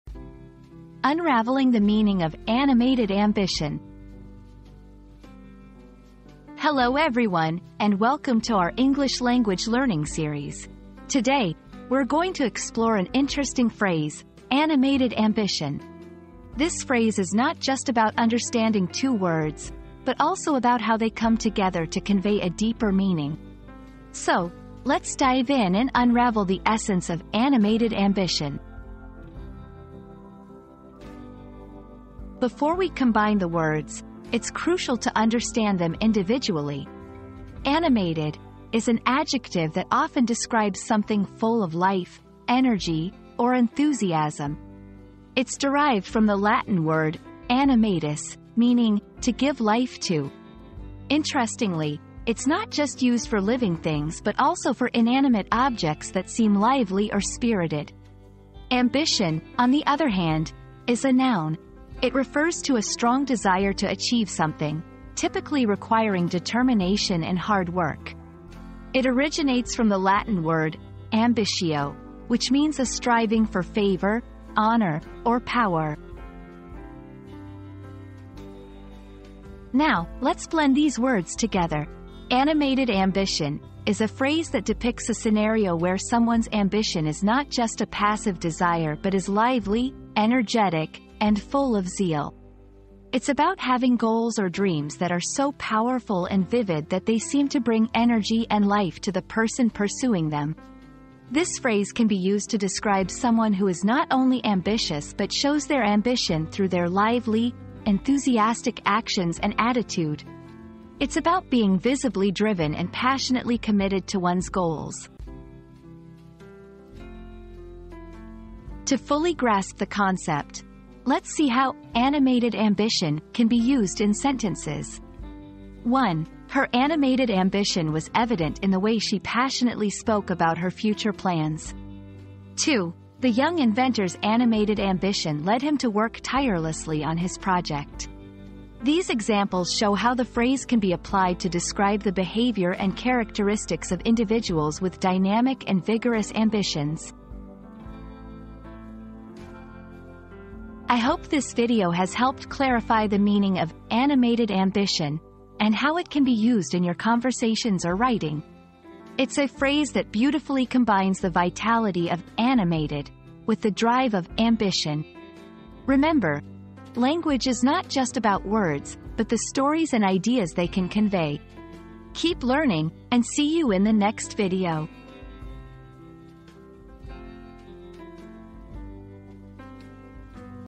Unraveling the Meaning of "Animated Ambition"
Unraveling the Meaning of 'Animated Ambition' • Discover the true essence and depth behind the concept of 'Animated Ambition'. Join us on a captivating journey as we delve into the intricacies and significance of this powerful phrase, exploring its impact on creativity, motivation, and personal growth. Get ready to unravel the secrets behind 'Animated Ambition' and unlock your full potential!

00:00 • Introduction - Unraveling the Meaning of "Animated Ambition"
00:31 • Understanding the Words: "Animated" and "Ambition"
01:19 • The Fusion - "Animated Ambition"
01:58 • Usage in Sentences